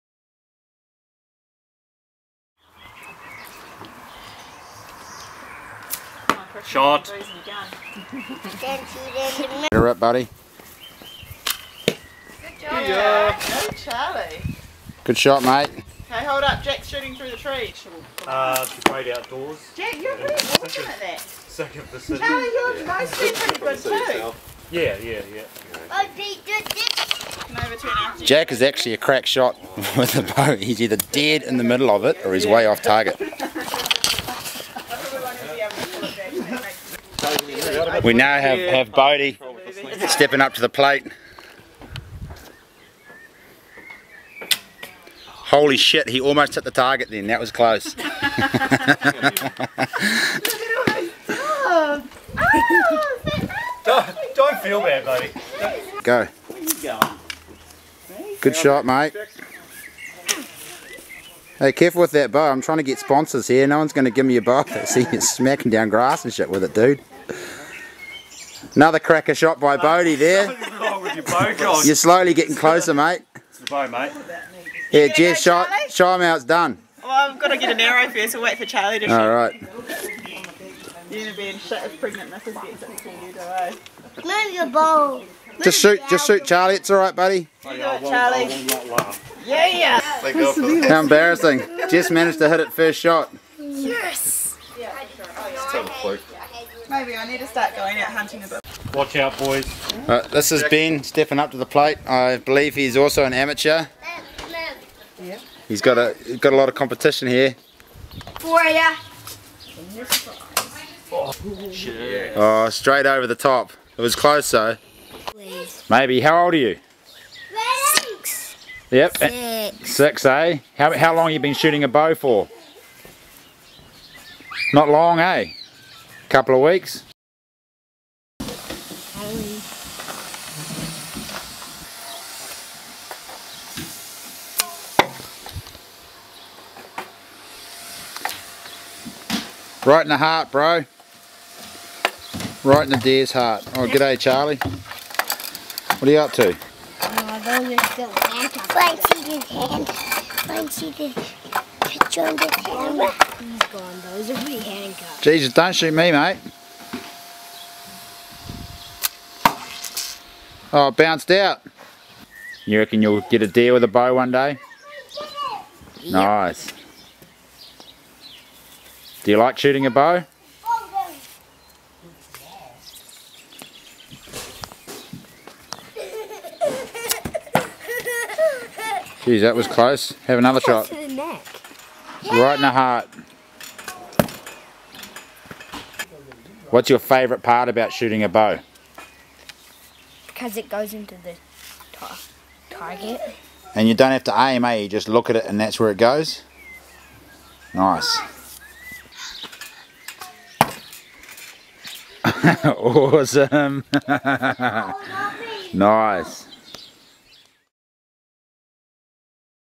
FATHER SON ADVENTURE Jack, 6 year old crack shot with a bow
FATHER SON ADVENTURE with Jack and Charlie of the bush